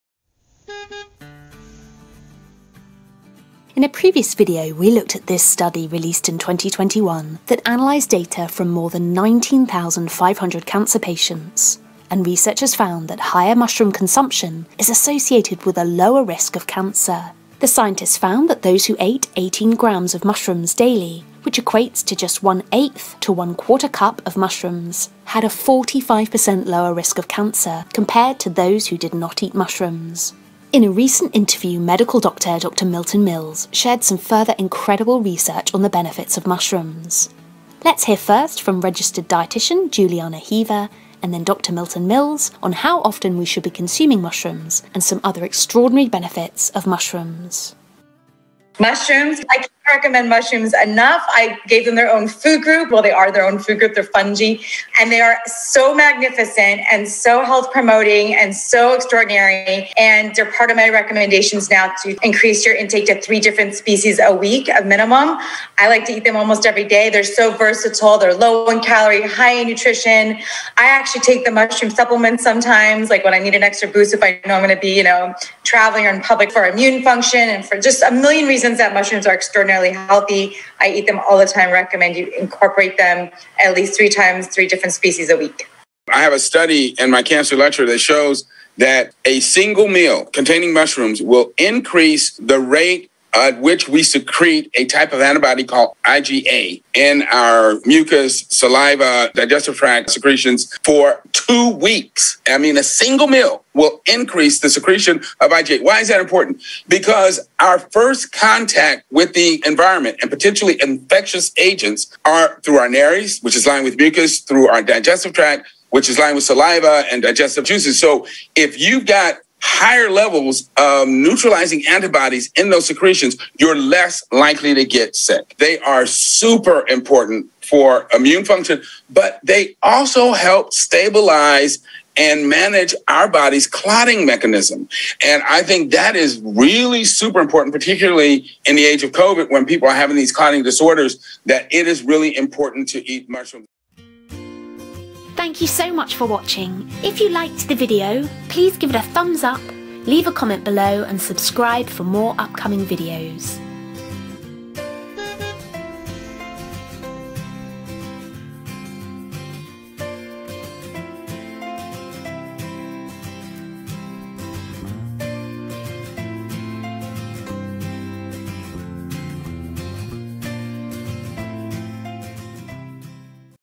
Single Meal Of Mushrooms, Effects Last 2 Weeks!!!
Mushrooms are incredibly healthy but how often should we eat them? Should we vary the different types of mushrooms we eat? Can just a single serving of mushrooms have effects that last 2 weeks? Why are mushrooms so powerful for immunity? Let's find out!

Higher Mushroom Consumption Is Associated with Lower Risk of Cancer: A Systematic Review and Meta-Analysis of Observational Studies

Dietary intake of Agaricus bisporus white button mushroom accelerates salivary immunoglobulin A secretion in healthy volunteers